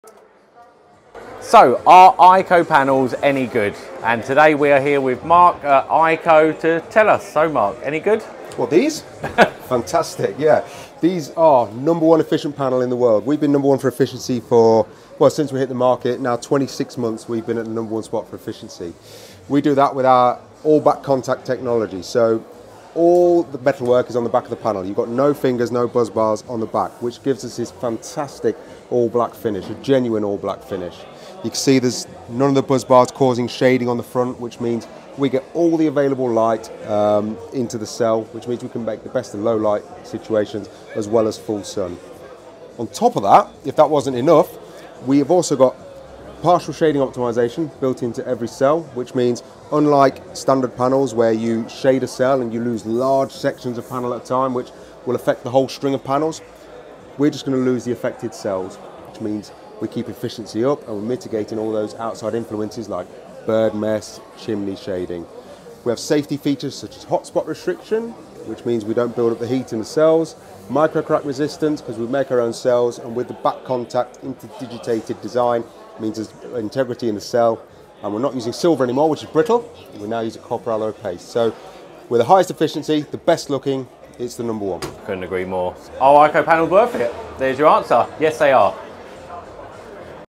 🔍 Are Aiko Solar Panels Any Good? Let’s Find Out! ☀️💡
We Caught up with Marc Eastgate - Business Development Manager at @AikoEnergy , to Unpack the Performance, Efficiency, and Innovation Behind one of the Most Talked-About Solar Panels on the Market.

Hit Play Now to See if Aiko Solar Panels Are Really Worth The Hype! ⚡